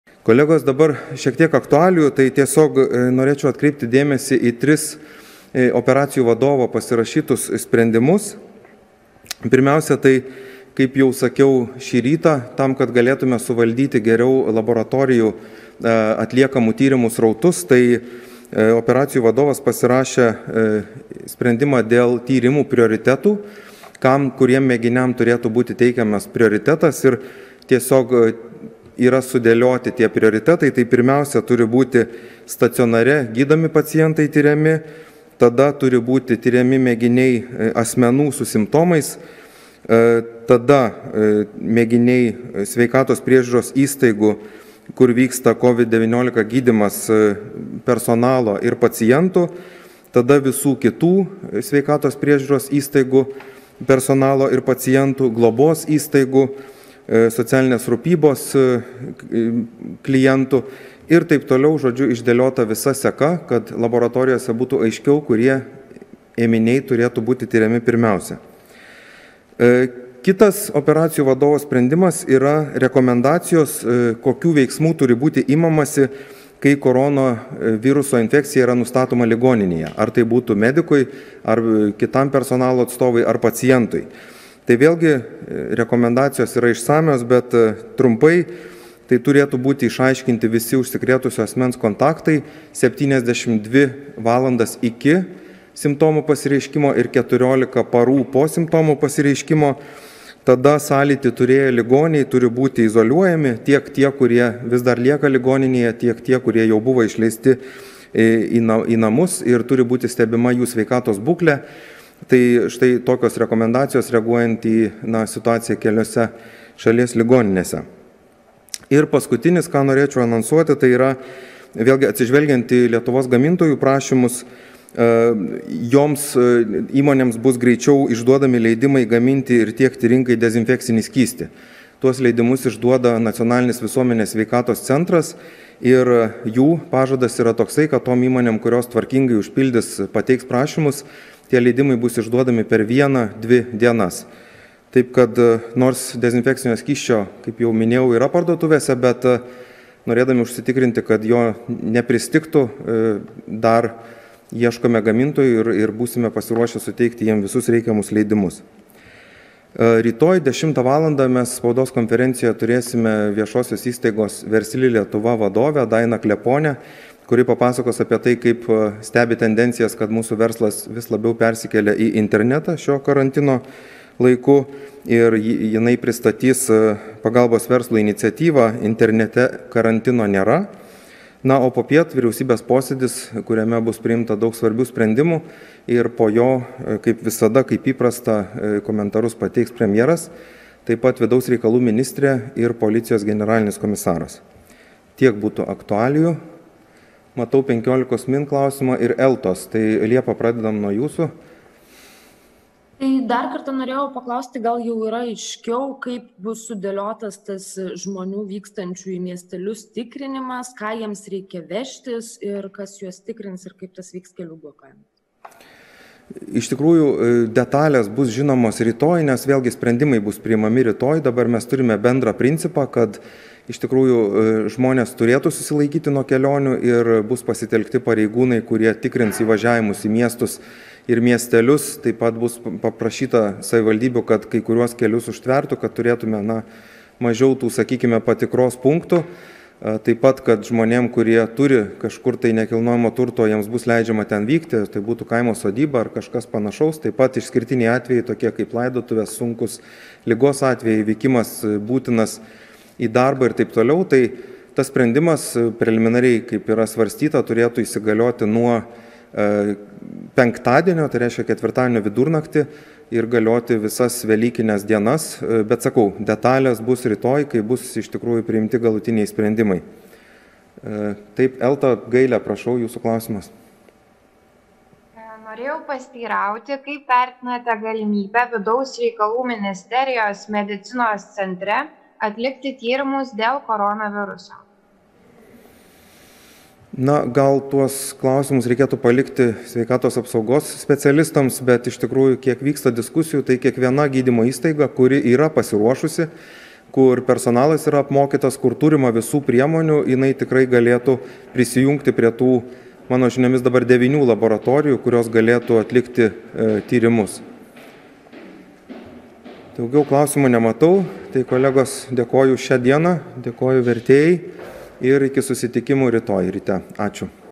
G. Surplys: judėjimo apribojimai įsigalios nuo penktadienio
G. Surplys pristatė antradienį pasirašytus sprendimus, susijusius su koronaviruso valdymu.